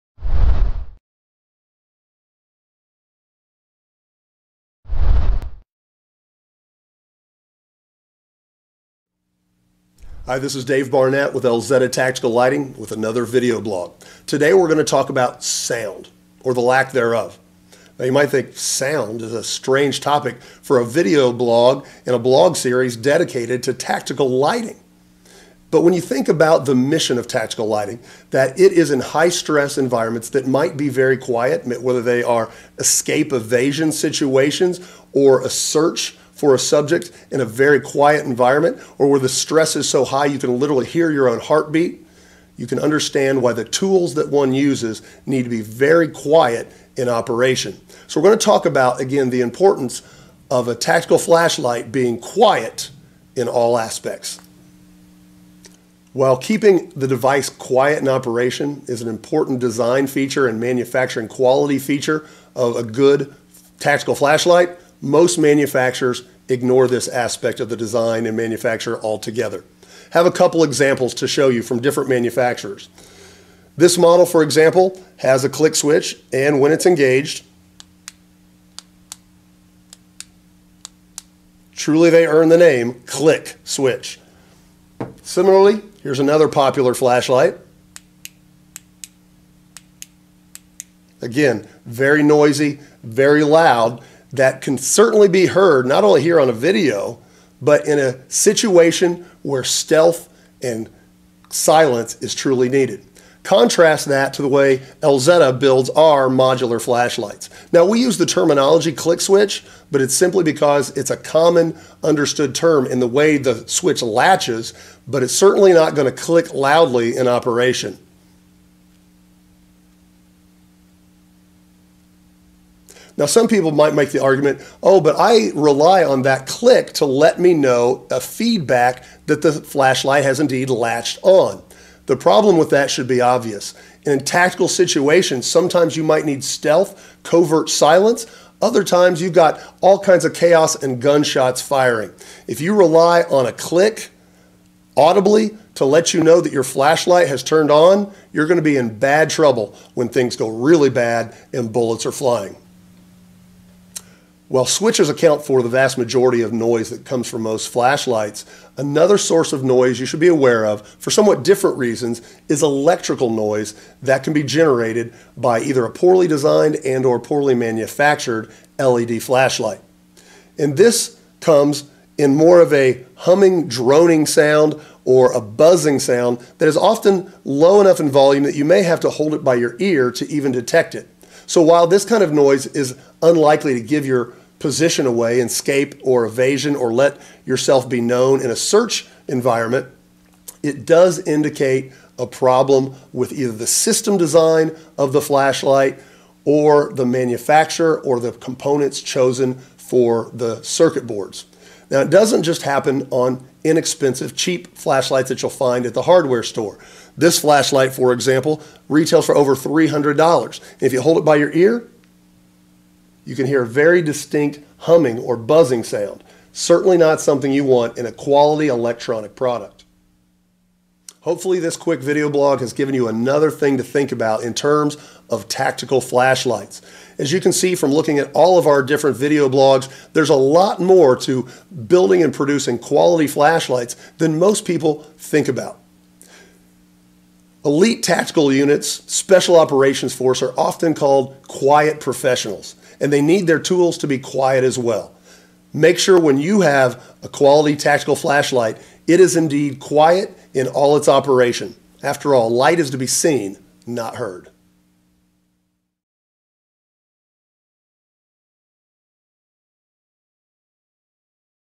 The Sound of Silence in Tactical Flashlights, Elzetta vlog episode 21
Loud, clunky switches have no place on a tactical flashlight.  Electrical noise generated by LED flashlights is also a problem.  This video blog addresses these sources of unwanted sound and explains why a quality tactical flashlight should be silent in all aspects of its operation.